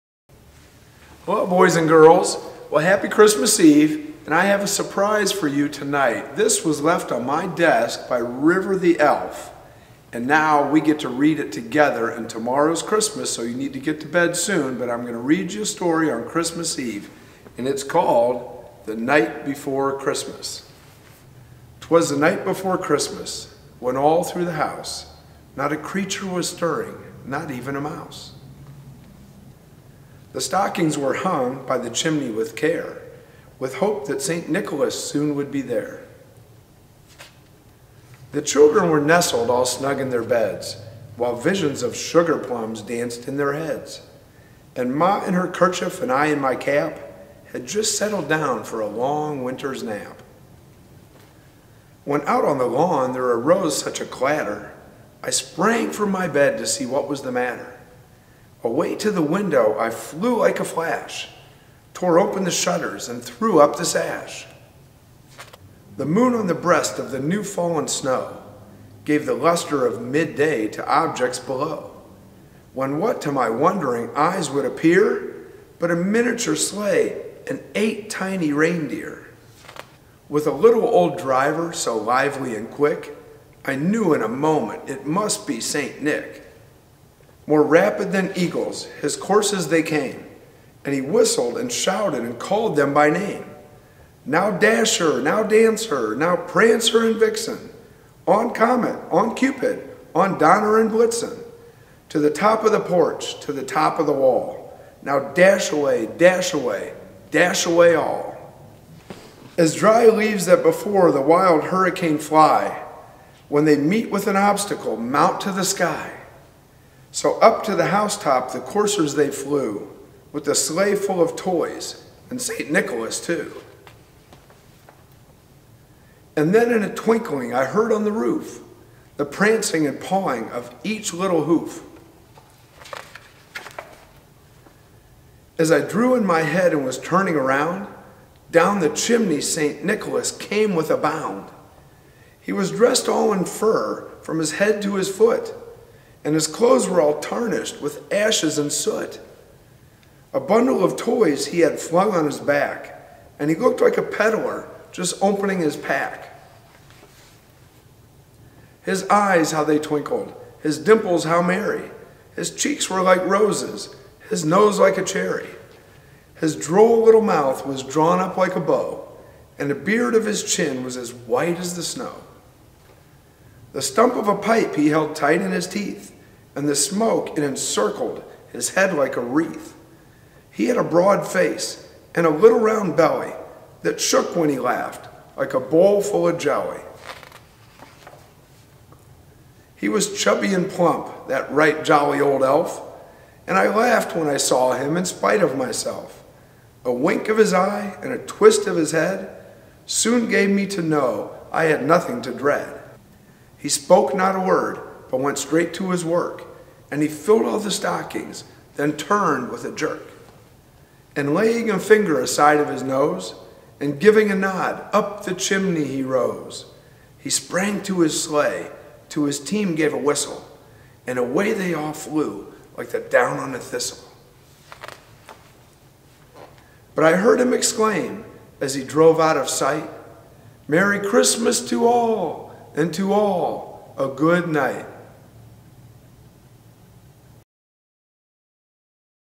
Night Before Christmas
Mayor Don Walters reads The Night Before Christmas